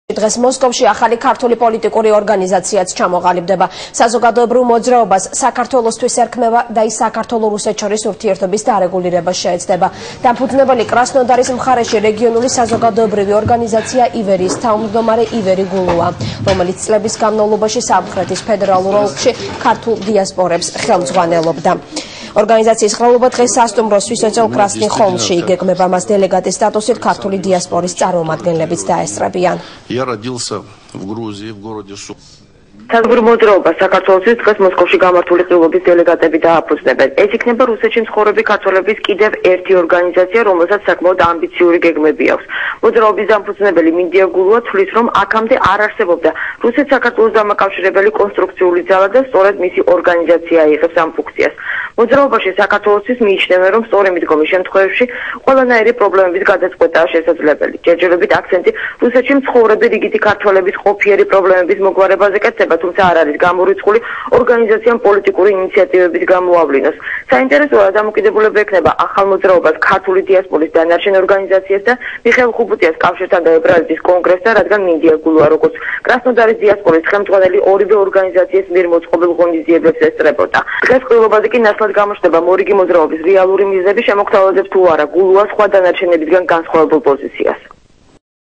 26.03.2010
მოსკოვში ახალი ქართული პოლიტიკური ორგანიზაცია ჩამოყალიბდა